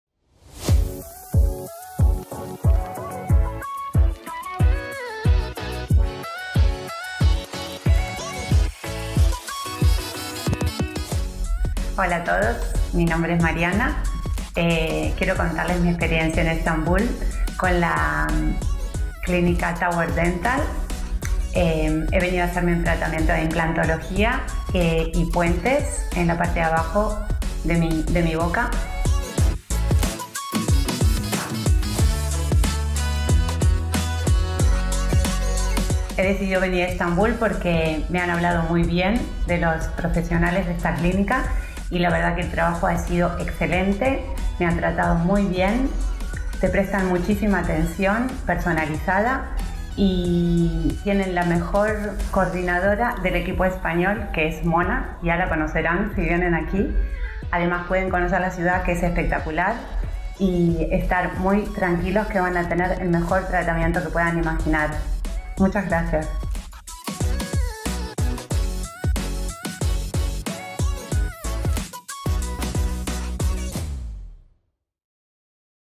3 Implantes 8 Coronas y Blanqueamiento Láser | Mariana de España - Tower Dental Clinic
Nuestra paciente Mariana de España.  Ella llegó a Tower Dental clínica Istanbul Turquía para obtener su tratamiento dental: 3 implantes, 11 coronas de circón y limpieza, además de un láser de blanqueamiento.  Quedó muy contenta con el resultado, y eso nos hace aún más felices.  Mire este video de Mariana hablando sobre su experiencia con Tower Dental Clinic, diciéndonos lo que piensa.

Ataköy 7-8-9-10, Ataköy Towers 7-8-9-10. Kısım Mah. E-5 No:20 B blok 7. Kat No:104, 34158 Bakırköy/İstanbul